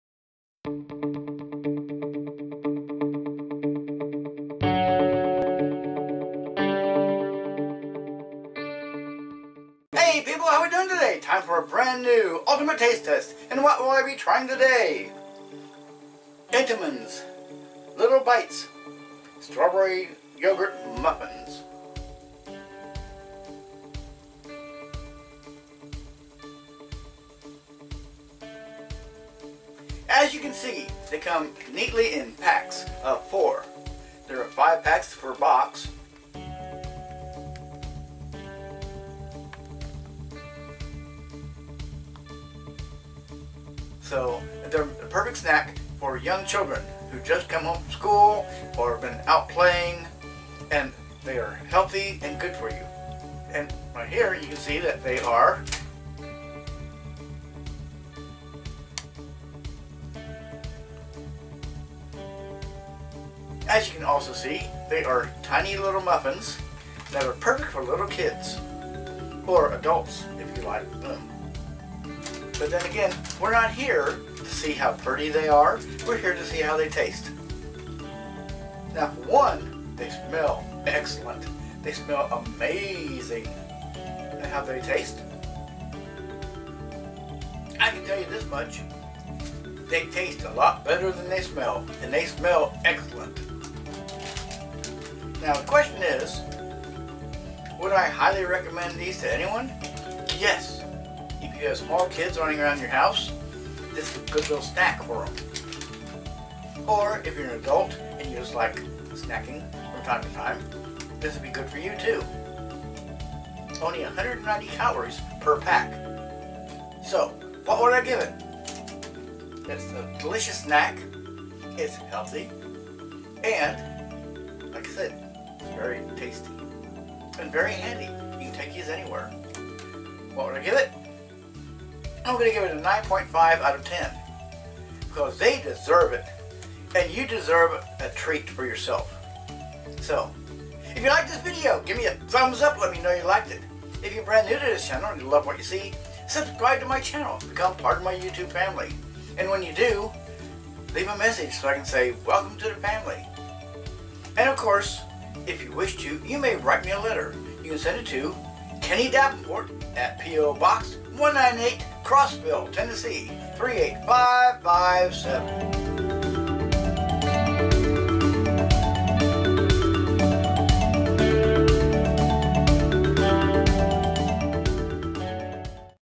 Ultimate Taste Test, Entenmann's Little Bites
On today's Ultimate Taste Test, I sample Entenmann's Little Bites, Strawberry Yogurt Muffins. These tasty muffins are baked with real ingredients like real strawberries, and with No high fructose corn syrup.
Bimbo Bakeries USA, Inc.